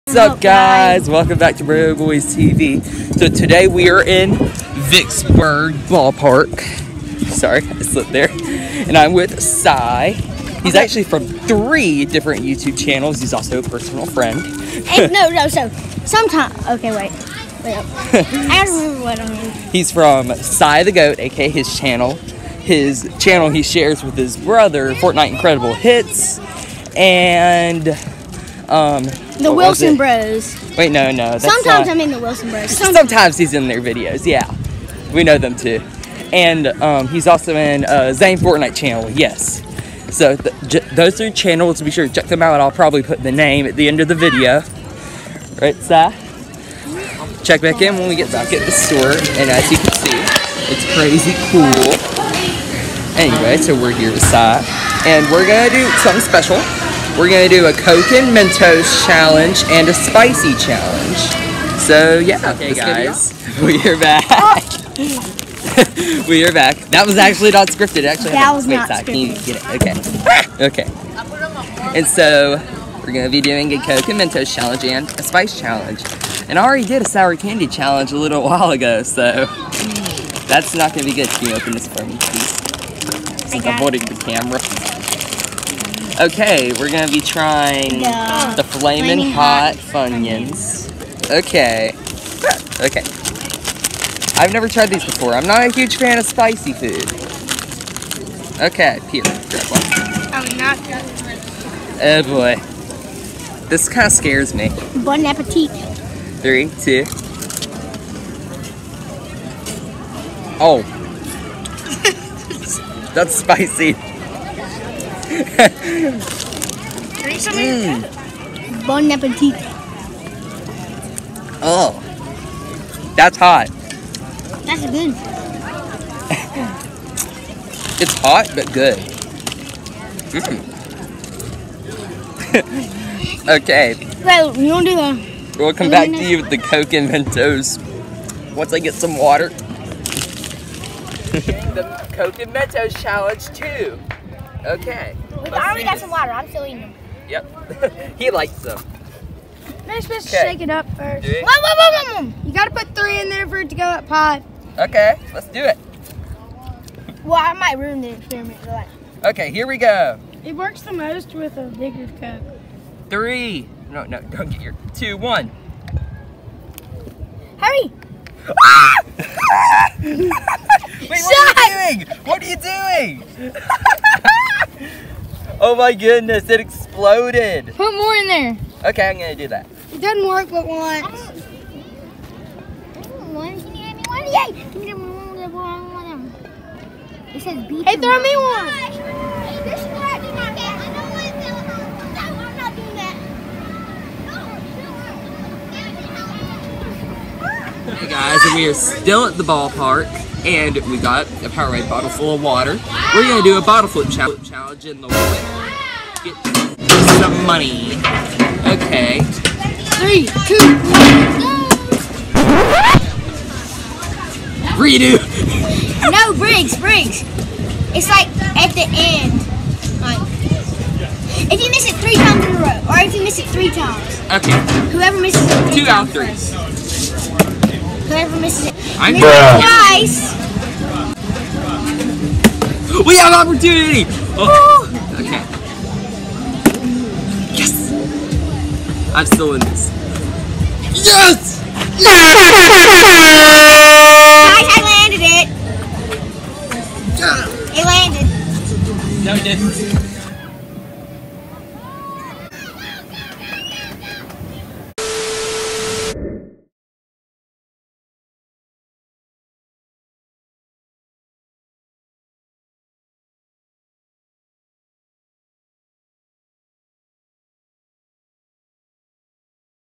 Awesome day with Cy at Vicksburg Sports Force ball park! Coke & Mentos, spicy food, and more!
Me and My friend Cy’s brothers are playing baseball together at Vicksburg sports force ball park and we both happen to be Youtubers so we filmed an awesome video on an awesome day!